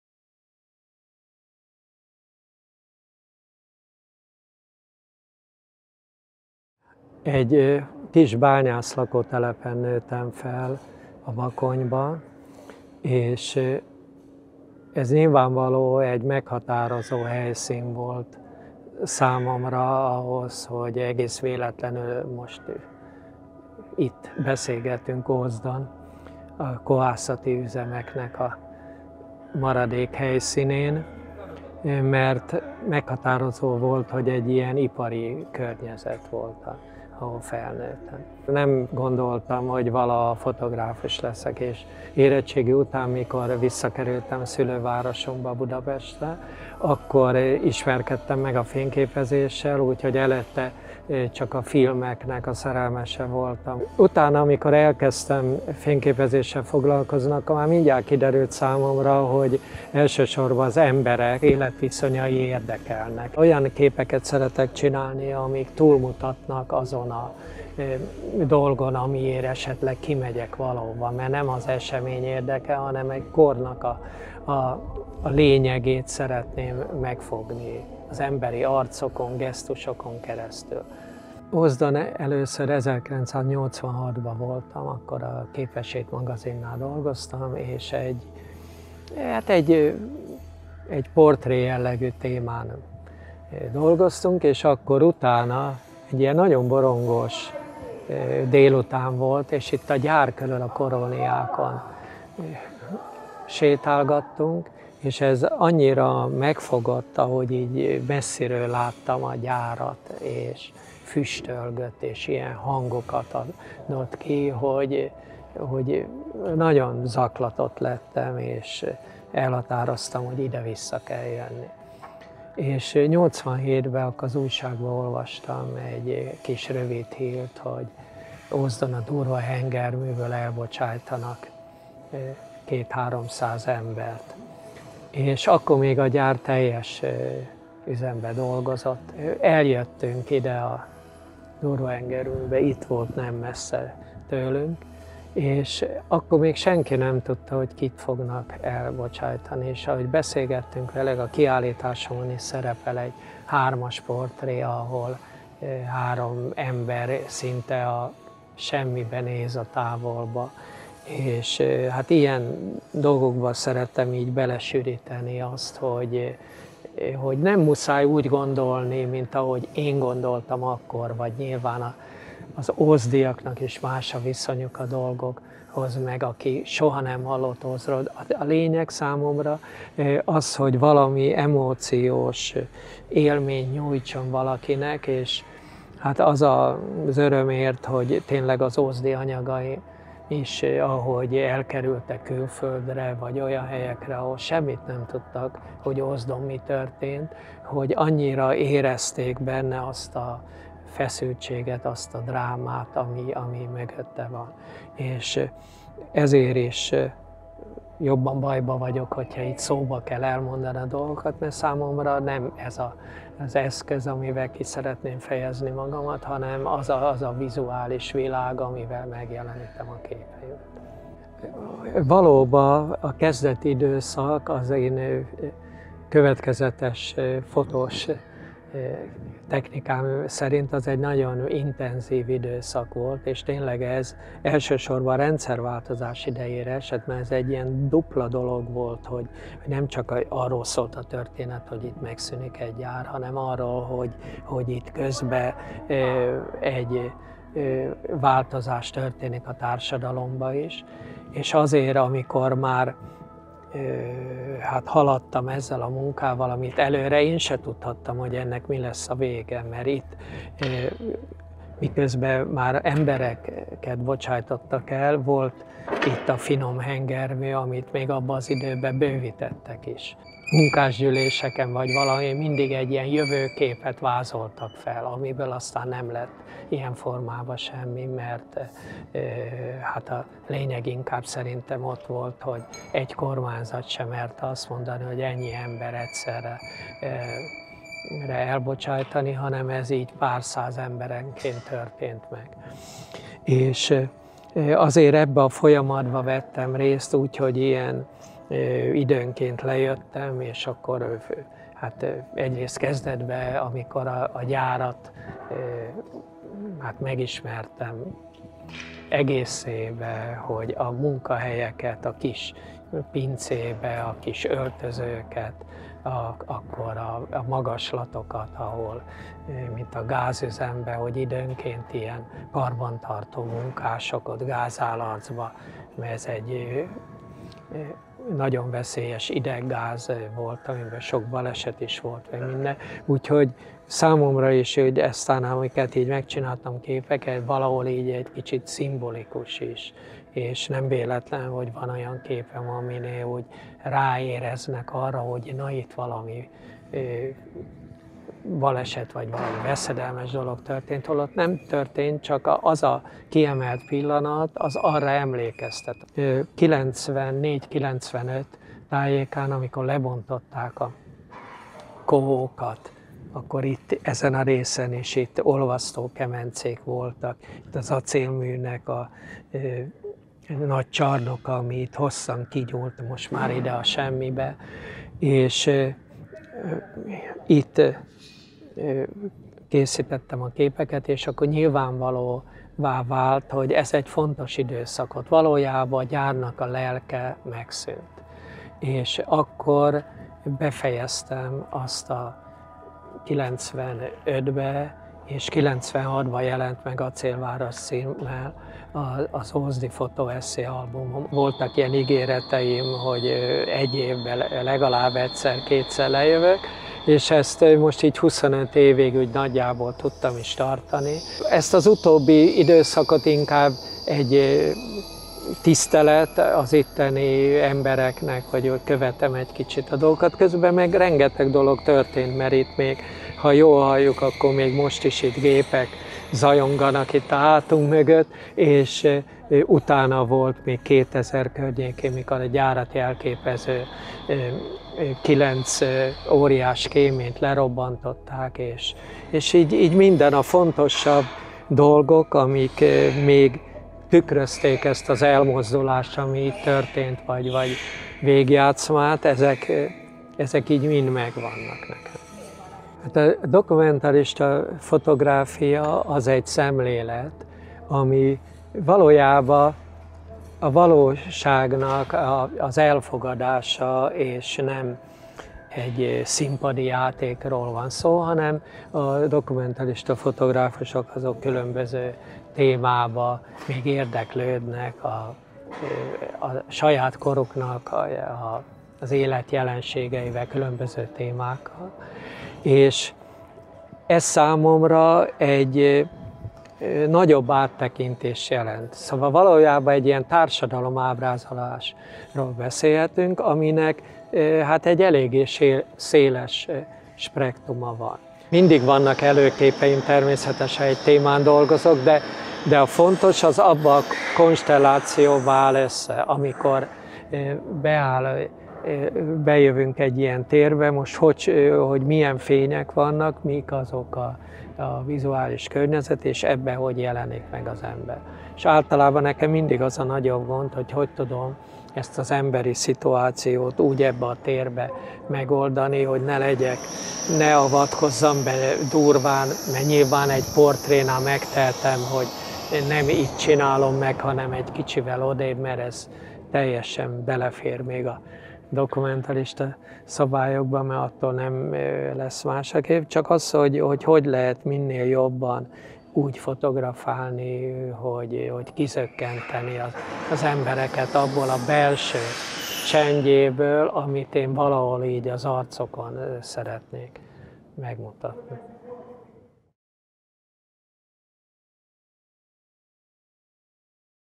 ACÉL - MŰ. Ózd, 1987–2014 – Beszélgetés Benkő Imre fotográfussal
Benkő Imre fotográfus, a Magyar Művészeti Akadémia rendes tagja tulajdonképpen az ózdi kohászat leépüléséről szóló fotóesszéjének köszönheti nemzetközi hírnevét. A kétszeres World Press Photo díjas, a dokumentarista fotográfia egyedi képi világú alkotója majd' 25 éven keresztül jár vissza pályájának meghatározó helyszínére, mai nevén a „Kultúrgyárba". „Az események alakulását előre nem láthattam, a hírek mögötti világ érdekelt. Az ember és környezetének sajátos bemutatásával a korszak vizuális lényegét, egy szubjektumon átszűrt életérzést szándékoztam közvetíteni." –írta katalógusában. Az ózdi Városi Művelődési Központ „Olvasó" Kisgalériájában ACÉL-MŰ. Ózd, 1987–2012 címmel nyílt kiállítása kapcsán beszélgettünk az alkotóval.